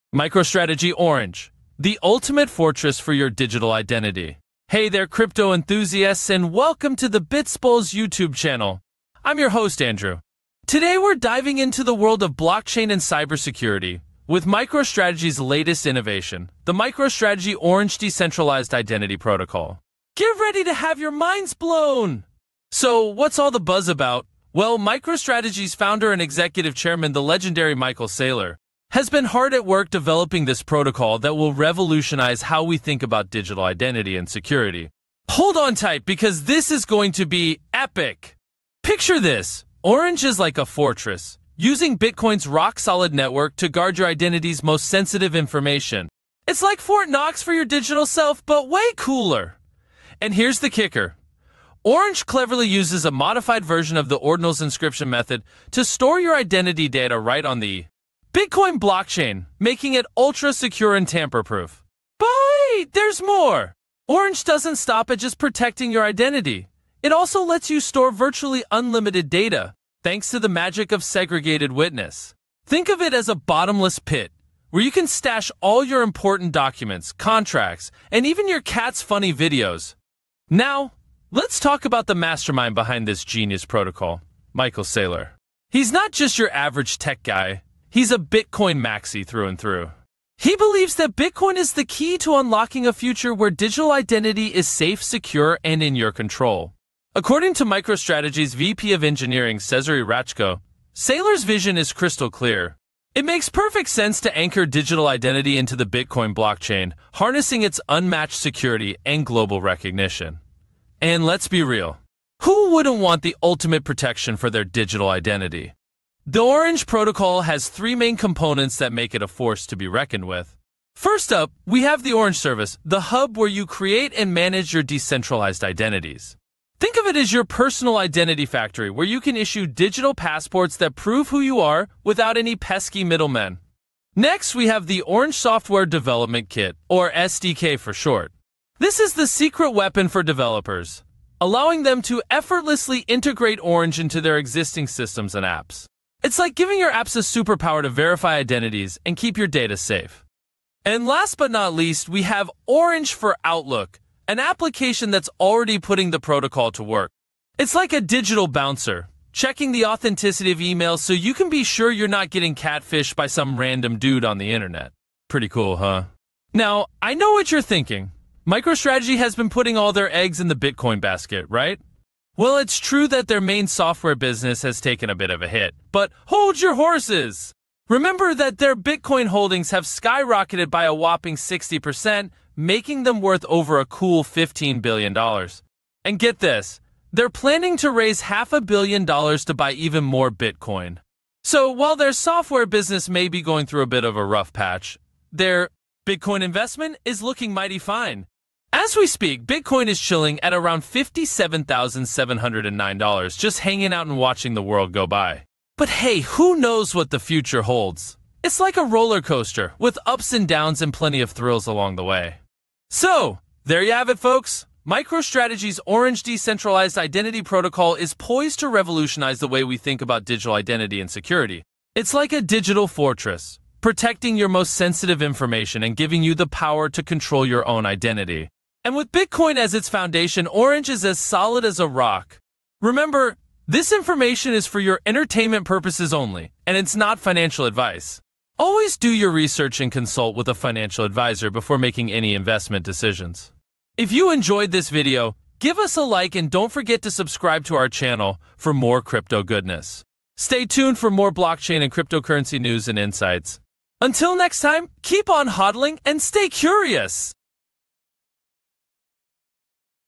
🤯MicroStrategy Orange: The Ultimate Fortress for Your Digital Identity 🛡️
💰💼 Bybit Copytrading 32+ million successful transactions, 75% with profit Earn money comfortably.

🚀 Dive into the future of digital identity and security with MicroStrategy's Orange protocol! 🛡️

In this video, we explore the revolutionary MicroStrategy Orange protocol that will change the game for blockchain and cybersecurity. 💻

✅ Key features:
- Rock-solid security with Bitcoin's network
- Store unlimited data on the blockchain
- Control your own identity without middlemen

🌟 Behind the scenes:
- Bitcoin maxi Michael Saylor's vision
- VP of Engineering Cezary Raczko on Orange's potential

🛠️ Components:
- Orange Service for creating identities
- Orange SDK for developer integration
- Orange for Outlook for email authentication

💰 MicroStrategy's investment:
- Billion-dollar Bitcoin holdings
- Plans to raise $500 million for more Bitcoin